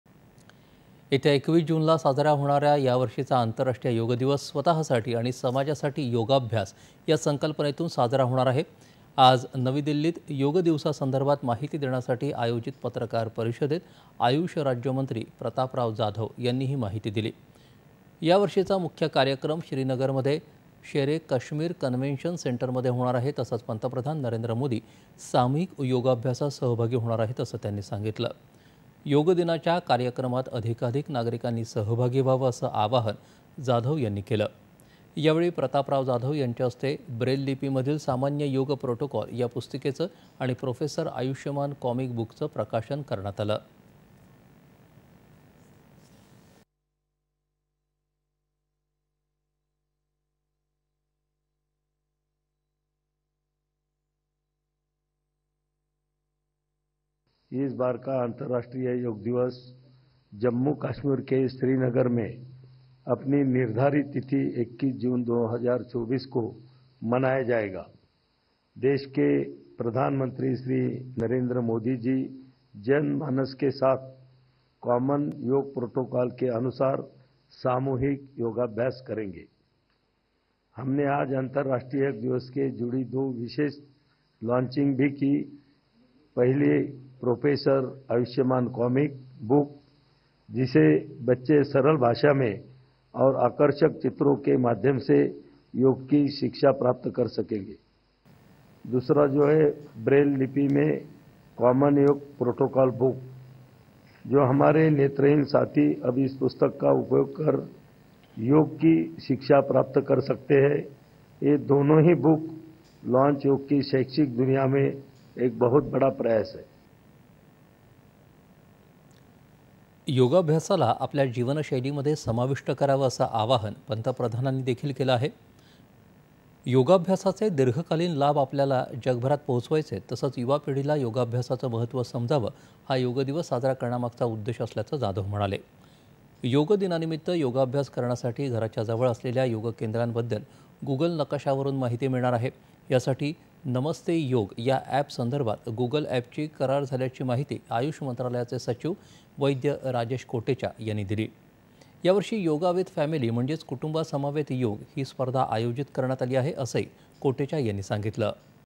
'स्वतःसाठी आणि समाजासाठी योगाभ्यास' या संकल्पनेतून साजरा होणार योग दिवस - प्रतापराव जाधव
दि. १८.०६.२०२४ :
  येत्या २१ जूनला साजऱ्या होणारा यावर्षीचा आंतराष्ट्रीय योग दिवस 'स्वतःसाठी आणि समाजासाठी योगाभ्यास' या संकल्पनेतून साजरा होणार आहे. आज नवी दिल्लीत योग दिवसासंदर्भात माहिती देण्यासाठी आयोजित पत्रकार परिषदेत, आयुष राज्यमंत्री  प्रतापराव जाधव यांनी ही माहिती दिली. यावर्षीचा मुख्य कार्यक्रम श्रीनगरमध्ये  शेर -ए - कश्मीर कन्व्हेन्शन सेंटरमध्ये होणार आहे तसंच पंतप्रधान नरेंद्र मोदी सामूहिक योगाभ्यासात सहभागी होणार आहेत, असं त्यांनी सांगितलं.  योग दिनाच्या कार्यक्रमात अधिकाधिक नागरिकांनी सहभागी व्हावं, असं आवाहन जाधव यांनी केलं. यावेळी प्रतापराव जाधव यांच्या हस्ते ब्रेल लिपी मधील सामान्य योग प्रोटोकॉल या पुस्तिकेचं आणि प्रोफेसर आयुष्यमान कॉमिक बुकचं  प्रकाशन करण्यात आलं.
सिंक .
प्रतापराव जाधव , आयुष राज्यमंत्री.
 योगाभ्यासाला आपल्या जीवनशैलीमध्ये समाविष्ट करावं, असं आवाहन पंतप्रधानांनी देखील केलं आहे. योगाभ्यासाचे दीर्घकालीन लाभ आपल्याला जगभरात पोहोचवायचे आहेत तसंच युवा पिढीला योगाभ्यासाचं महत्व समजावं हा योग दिवस साजरा करण्यामागचा उद्देश असल्याचं जाधव म्हणाले.
 योग दिनानिमित्त योगाभ्यास करण्यासाठी घराच्या जवळ असलेल्या योग केंद्रांबद्दल गूगल नकाशावरून माहिती मिळणार आहे. यासाठी नमस्ते योग या ऍपसंदर्भात, गूगल ऍपची करार झाल्याची माहिती आयुष मंत्रालयाचे सचिव, वैद्य राजेश कोटेचा  यांनी दिली. यावर्षी योगा विथ फॅमिली म्हणजेच कुटुंबासमवेत योग ही स्पर्धा आयोजित करण्यात आली आहे, असंही कोटेचा यांनी सांगितलं.

8.30am | 11.00am | 1.00pm | 4.30pm | 7.00pm | 9.30pm